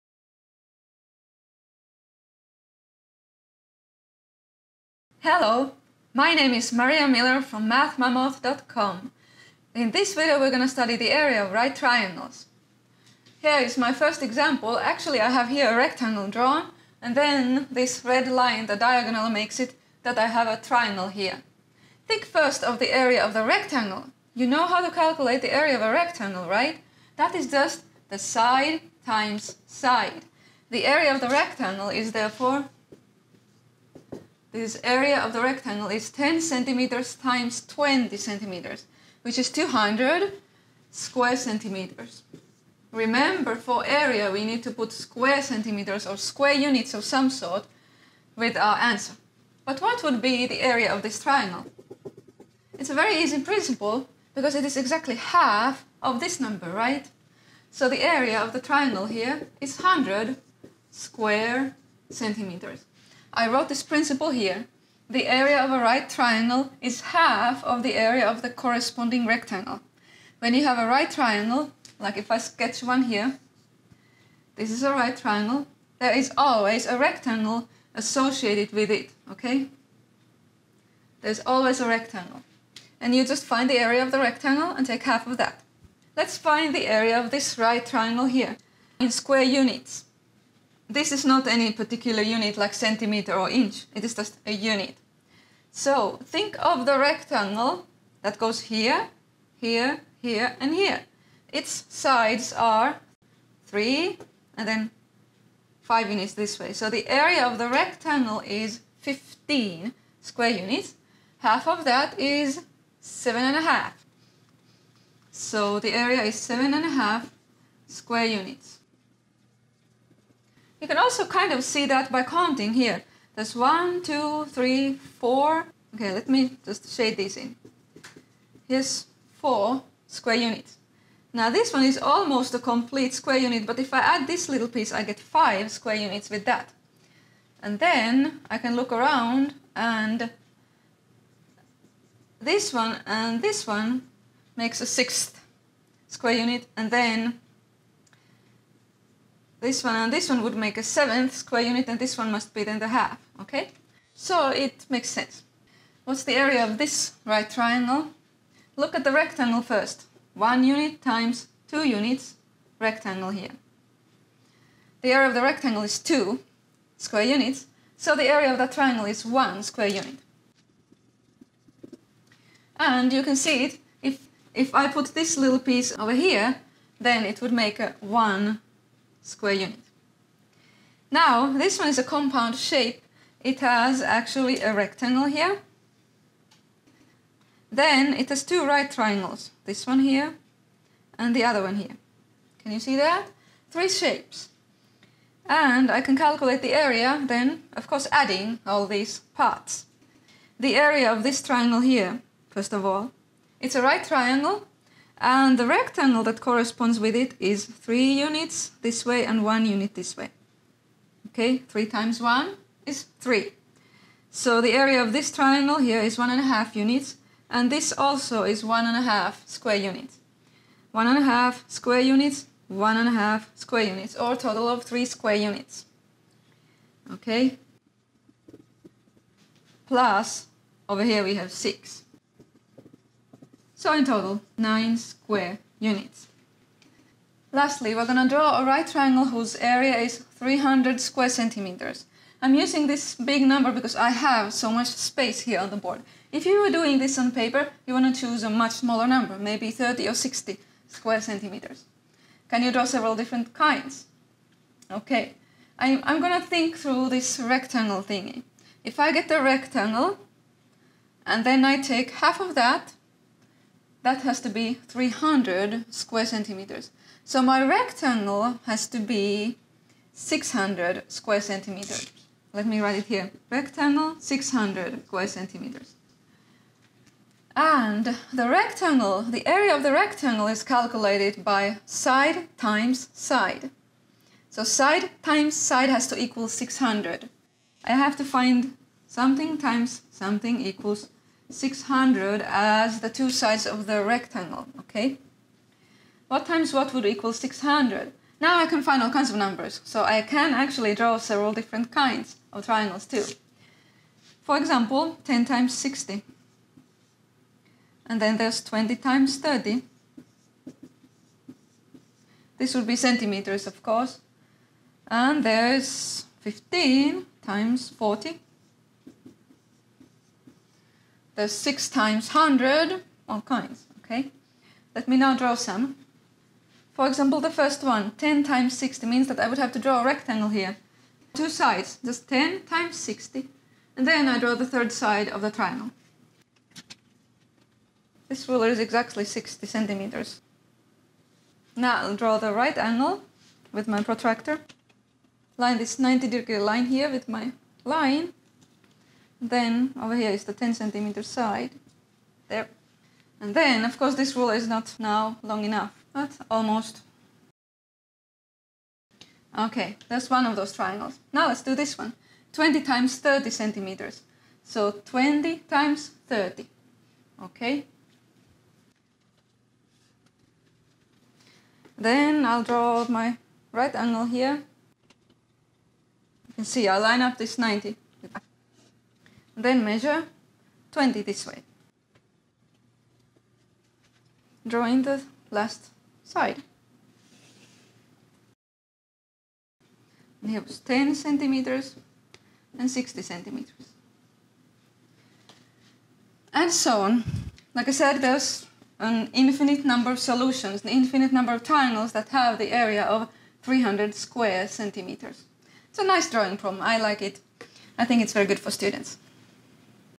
Area of right triangles
This is a lead-up lesson to the lesson about the area of triangles. In this lesson we only deal with RIGHT triangles. It is easy to see their area is HALF of the area of the rectangle drawn around the triangle.

We find the areas of some right triangles and the area of a compound shape. Lastly, the task is to DRAW a right triangle with a given area (300 square centimeters). I show how to find the base and the height for this triangle and draw 2 different ones.

In the follow-up lesson we learn how to find the area of any triangle:

This lesson is meant for 6th grade math.